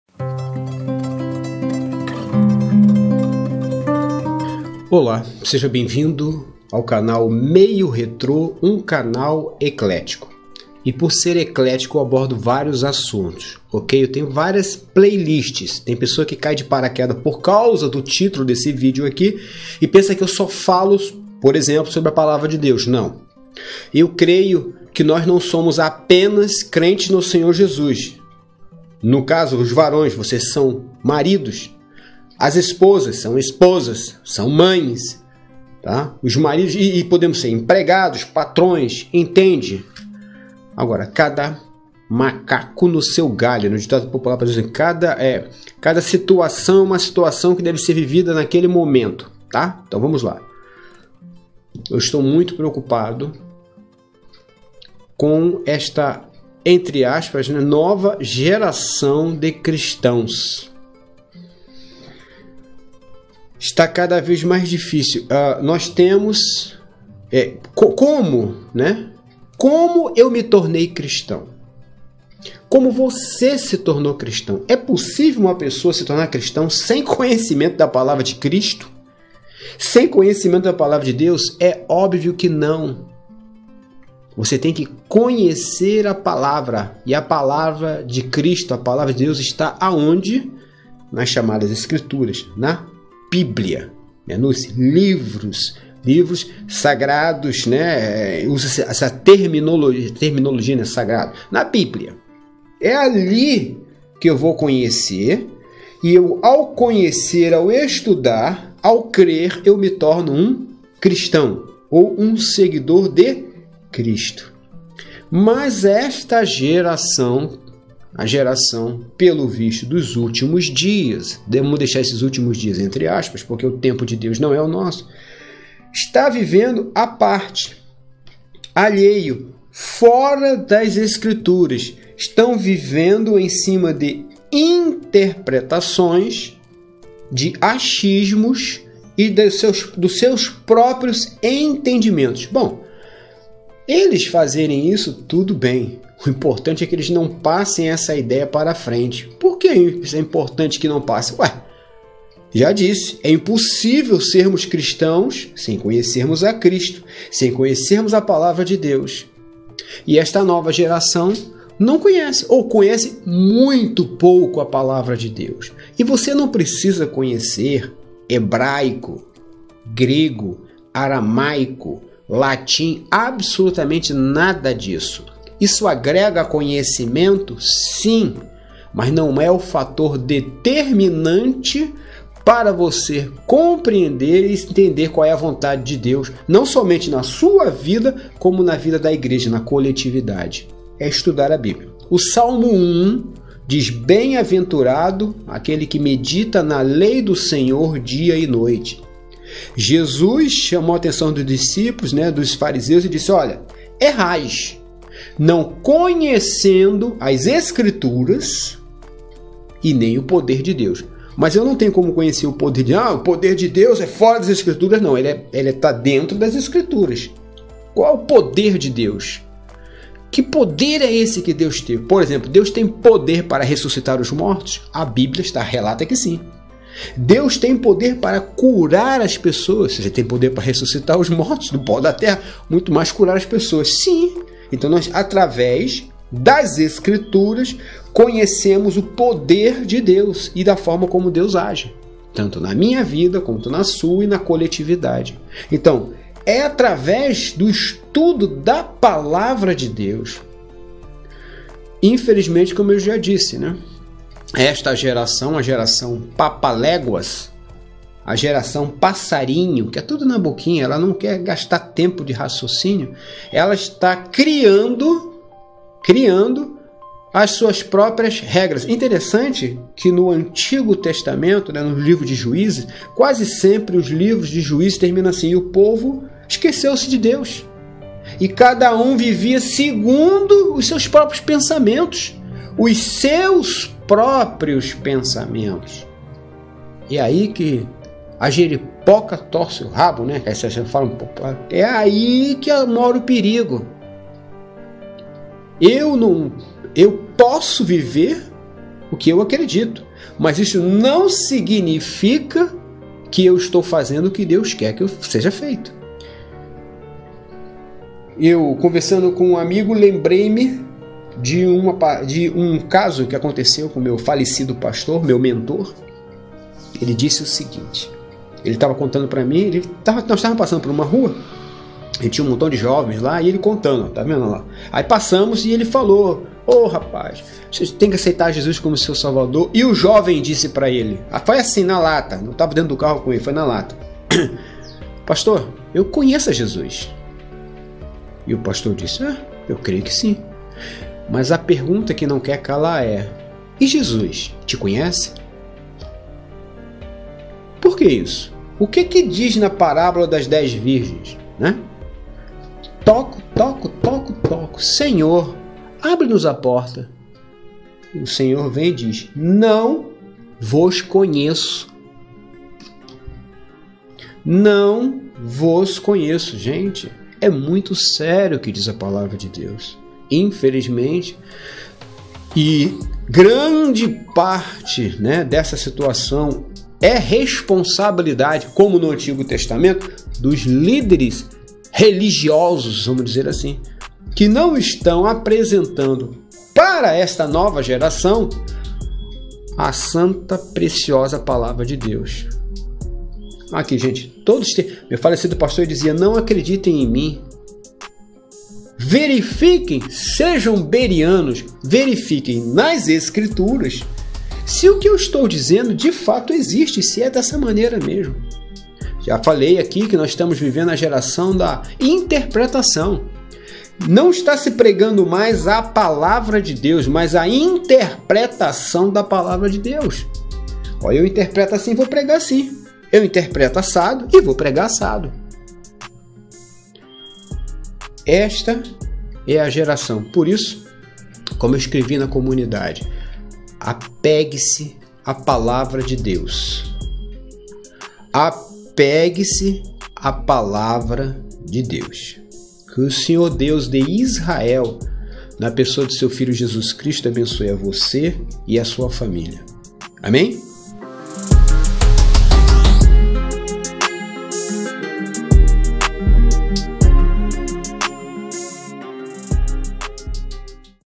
Apegue-se a PALAVRA DE DEUS
A cada dia que passa mais e mais "espantado" eu fico com essa nova geração de cristãos. Como nos tornamos cristãos? É possível ser cristão sem o conhecimento da Palavra de Deus? É claro que NÃO! Tudo o que eu sou como cristão é consequência do meu conhecimento da Palavra de Deus. Essa nova geração de cristãos estão vivento fora da Palavra, crendo que isso é possível, mas não é! Por isso eu lhes digo: SE APEGE A PALAVRA DE DEUS!